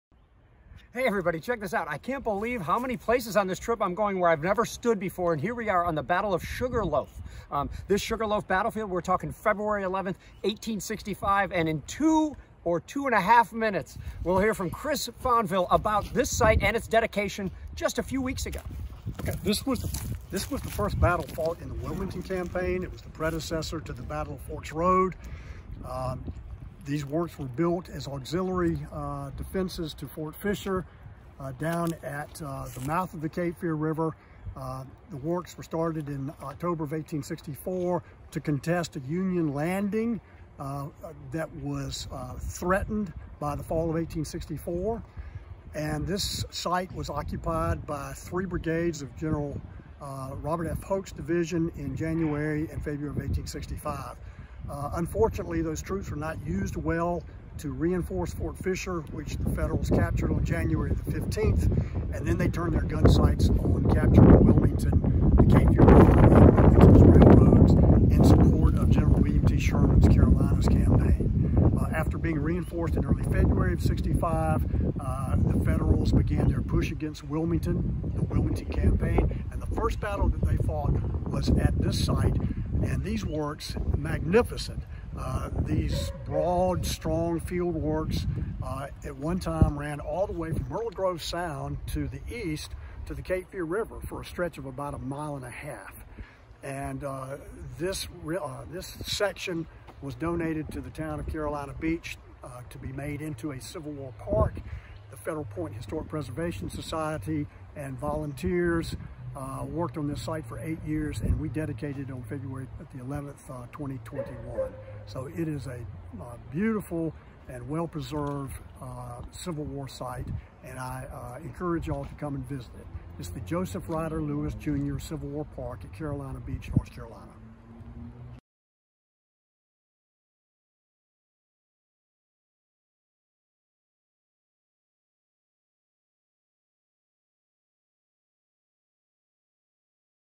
New Battlefield Park in Wilmington, NC, "Sugar Loaf": North Carolina Video Tour!
On February 11, 1865, the little known Battle of Sugar Loaf opened up the Wilmington Campaign. Join Chris Fonvielle Jr. for this brief tour of the brand new Joseph Ryder Lewis Jr. Civil War Park, dedicated to telling the story of those who fought throughout Wilmington, North Carolina.

This video is part of our Petersburg and North Carolina Battlefield Tour series.